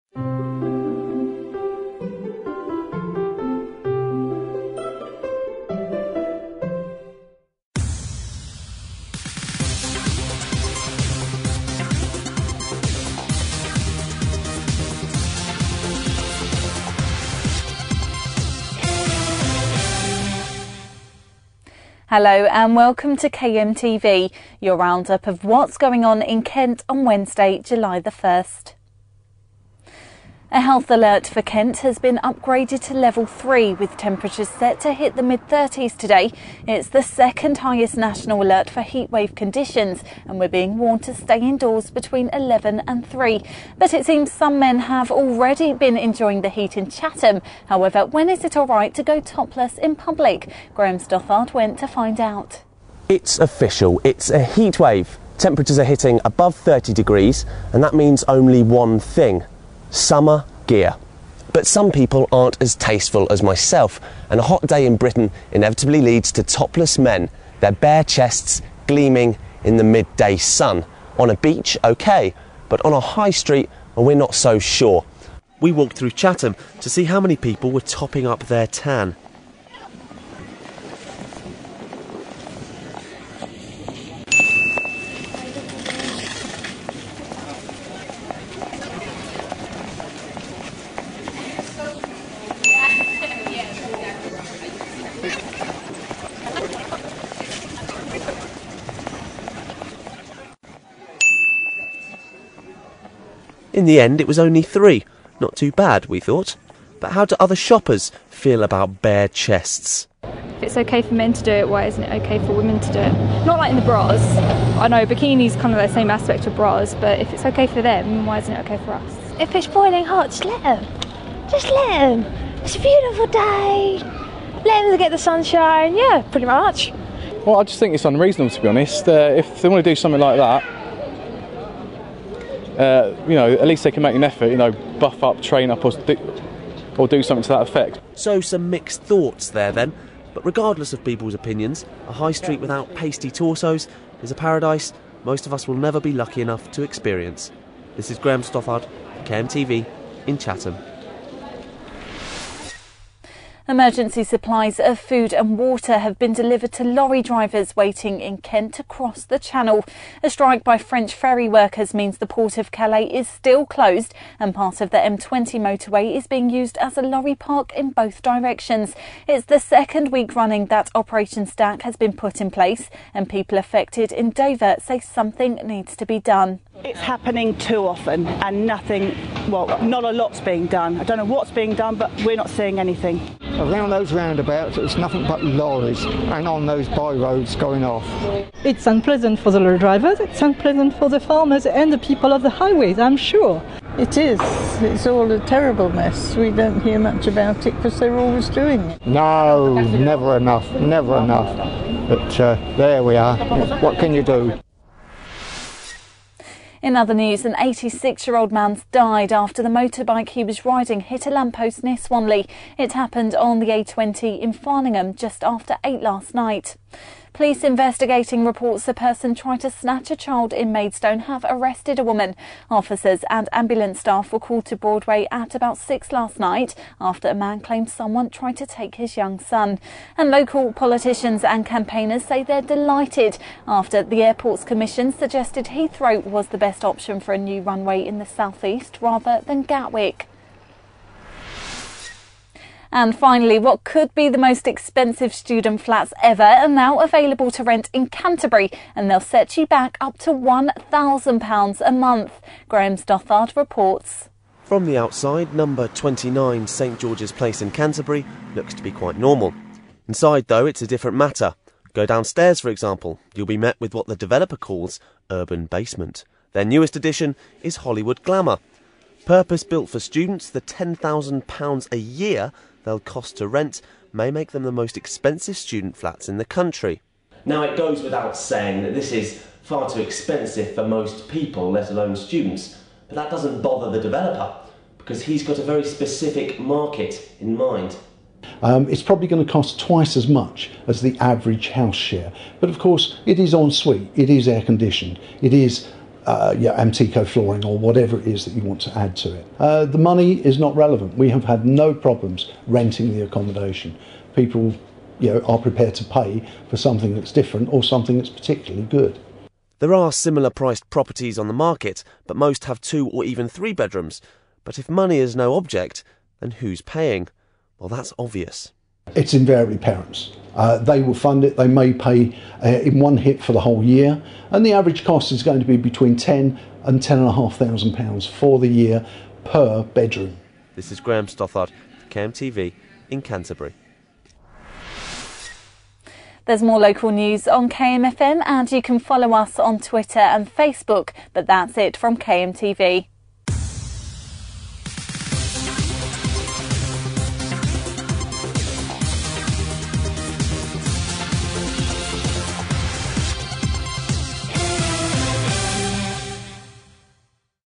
KMTV - Wednesday July 1
A round up of the top video news stories from around Kent.

On KMTV today: Kent's heatwave alert has been stepped up to level 3, the second highest, lorry drivers in Operation Stack are provided with food and water and student accommodation costing £1000 a month is now available in Canterbury.